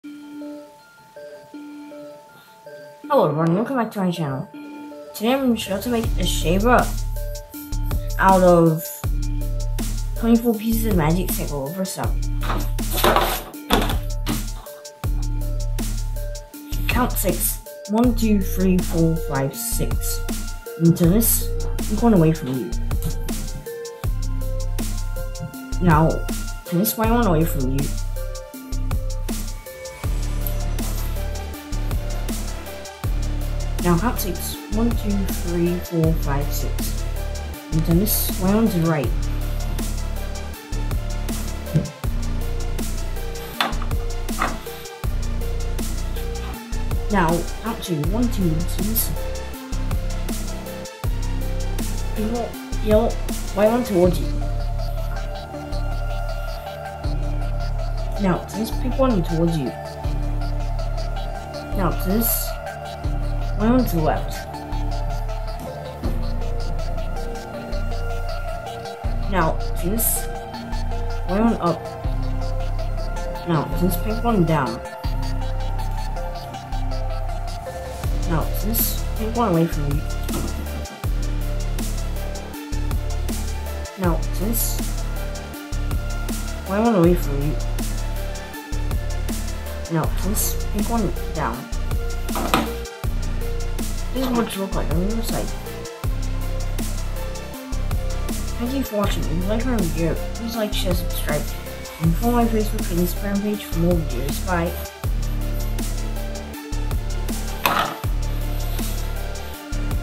Make A Shaver - 24 Pieces Magic Snake Puzzle
In this video, I will show you step by step How To Make A Shaver out of 24 Pieces of Magic Snake Puzzles. Hope you like my video!